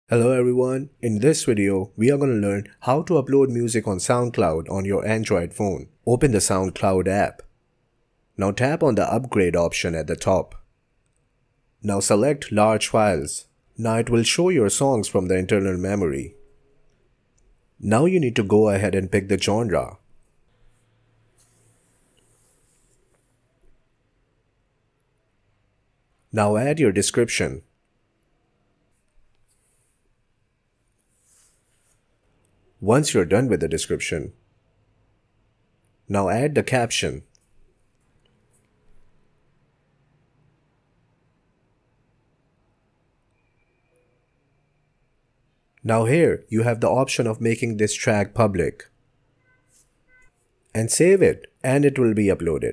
How To Upload Music To Soundcloud On Android (Quick)
In this tutorial, you will learn How To Upload Music To Soundcloud On Android in easy steps by following this super helpful tutorial to get a solution to your problem!!